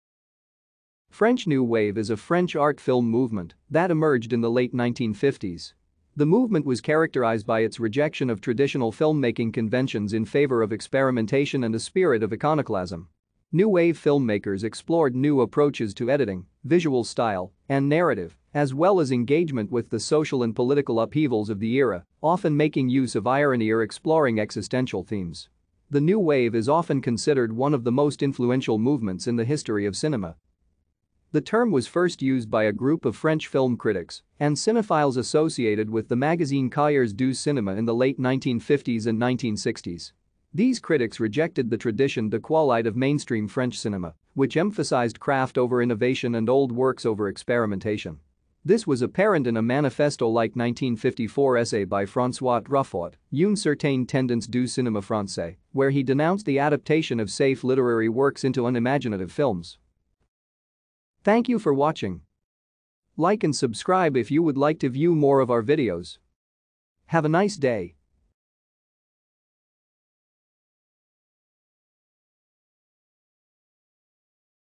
French New Wave
French New Wave  is a French art  movement that emerged in the late 1950s. The movement was characterized by its rejection of traditional making conventions in favor of experimentation and a spirit of iconoclasm. New Wave makers explored new approaches to editing, visual style, and narrative, as well as engagement with the social and political upheavals of the era, often making use of irony or exploring existential themes. The New Wave is often considered one of the most influential movements in the history of cinema.